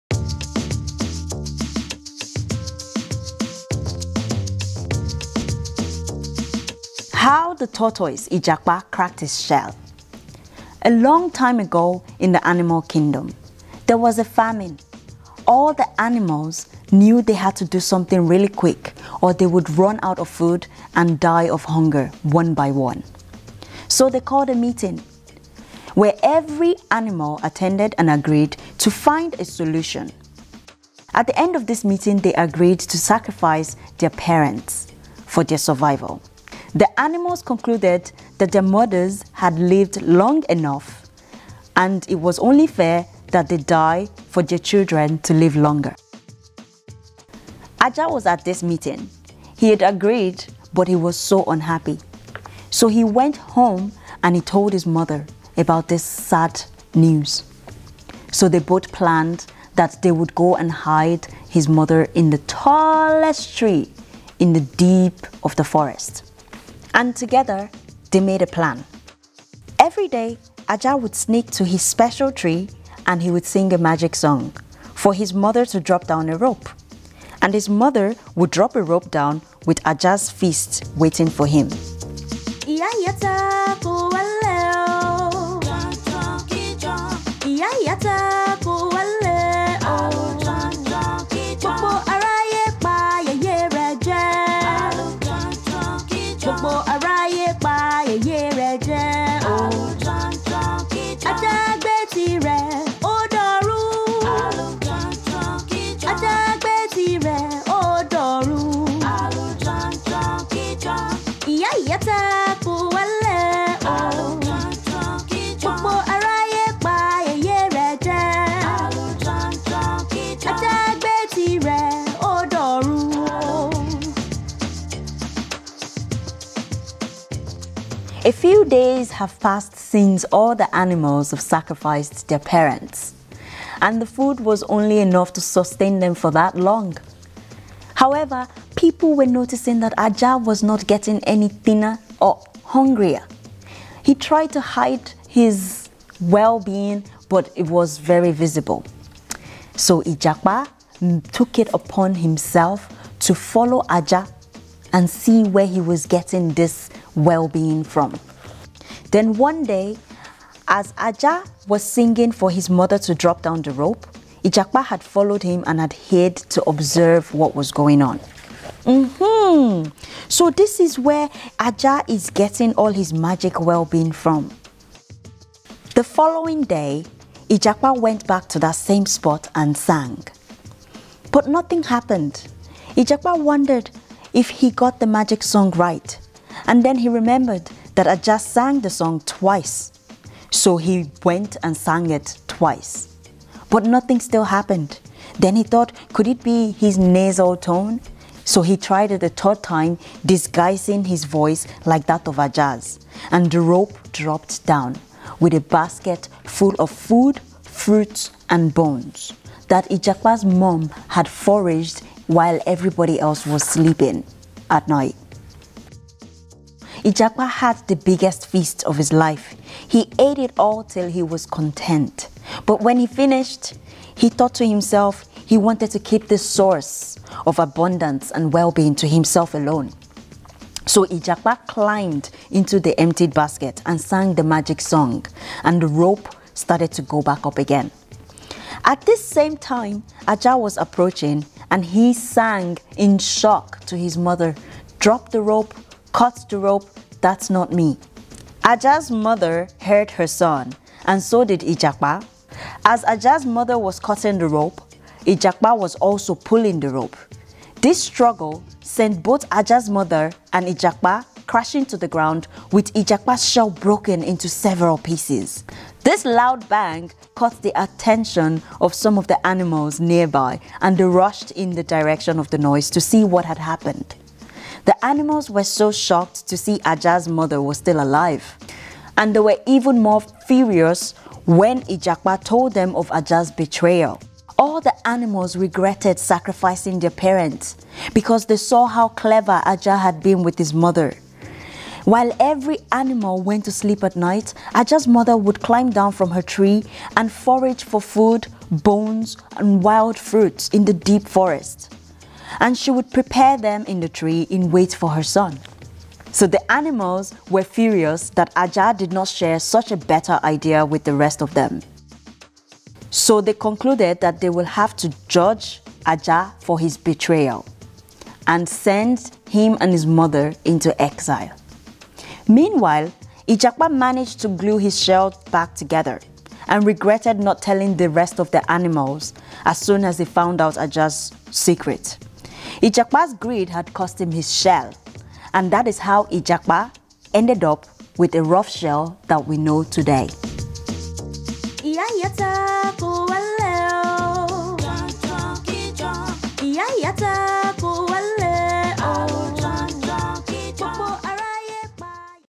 Yoruba bedtime stories and songs. How Íjàpá cracked his Shell
Mother Tongues Festival in collaboration with Singer and Songwriter Zeenie Summers presents a series of bed-time stories from the Yoruba tradition from Nigeria. Each night Zeenie will transport children and their families into a magical world, through beautifully crafted stories and songs.

This event is taking place as part of South Dublin County Council’s Social Inclusion Festival.